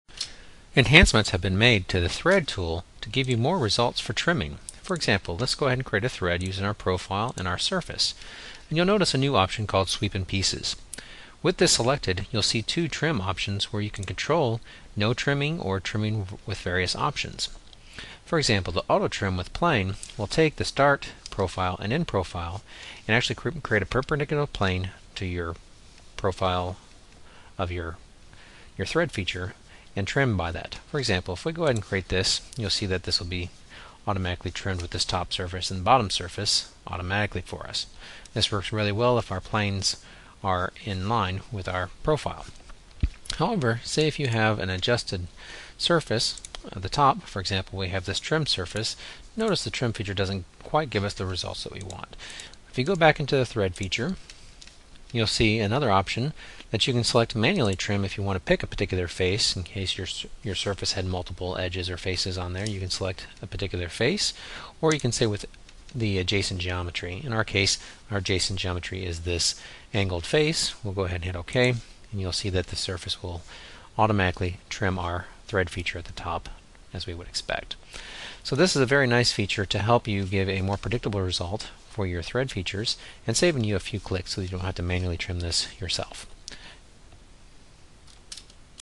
Thread Feature Trim Options in IronCAD
Settings to automatically trim the Thread Feature in IronCAD